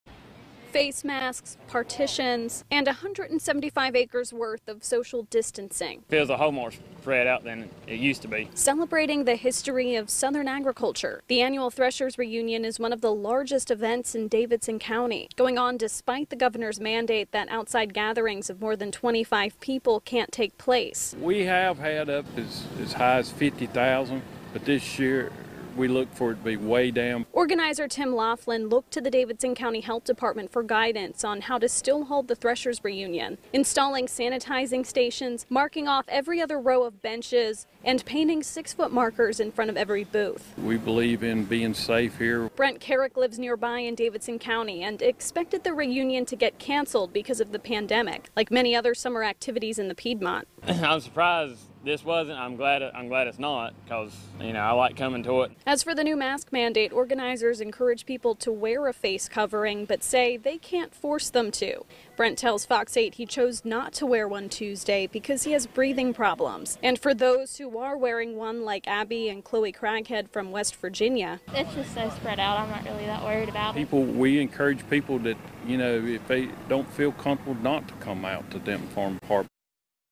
50th-annual Thresher's Reunion kicks off with safeguards in place
It's one of the largest tractor shows in the southeast and despite Gov. Roy Cooper's executive order that bans gatherings of more than 25 people, the 50th-annual Thresher's Reunion kicked off Tuesday morning at Denton FarmPark.